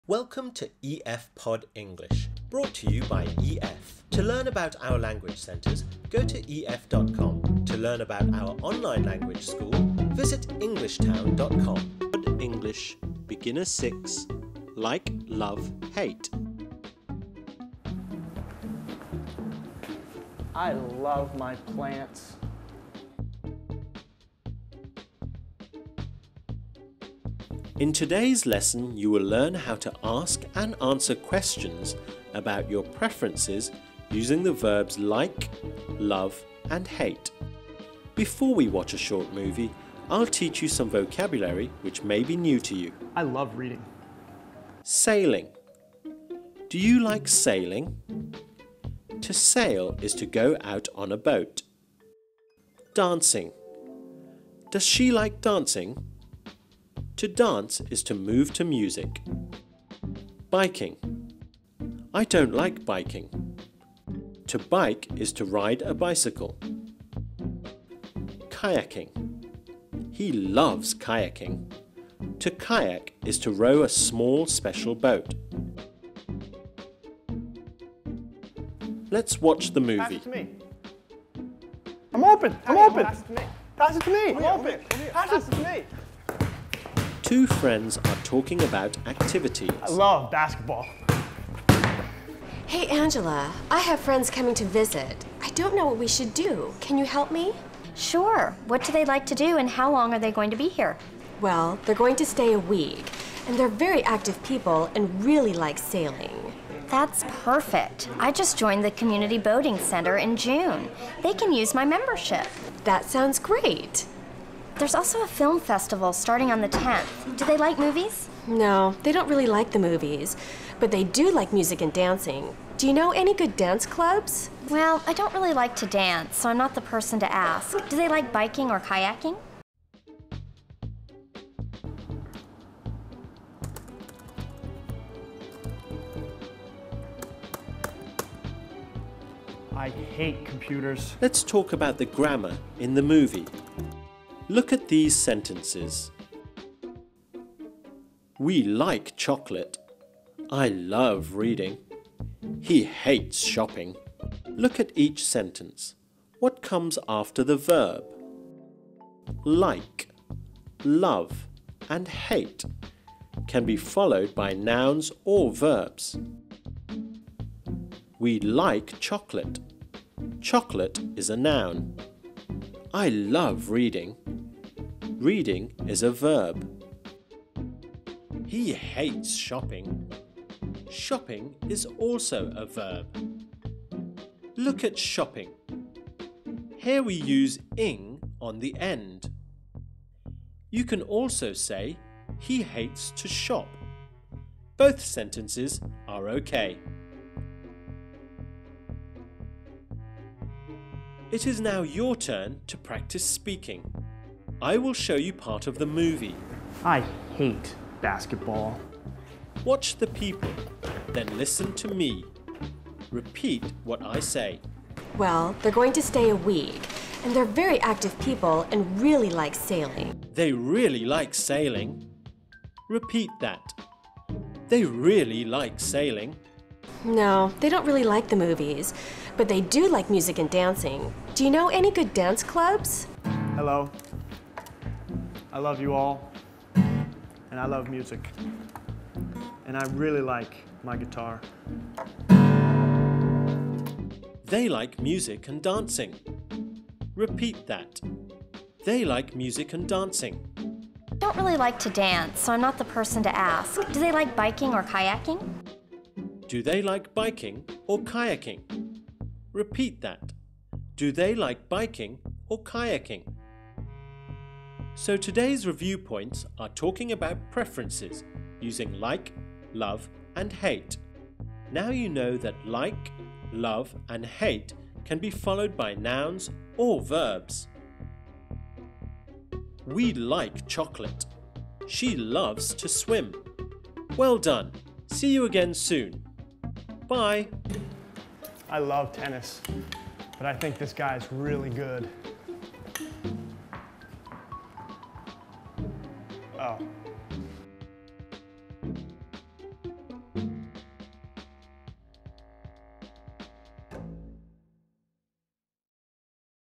Fun English Lesson 6 - Like, love, hate
In this beginner lesson you will learn the grammar to form simple sentences with the verbs "like", "love" and "hate" in English. You will also learn some vocabulary for sports and activities that you may enjoy.
Curious about learning online with Englishtown.com? for more information!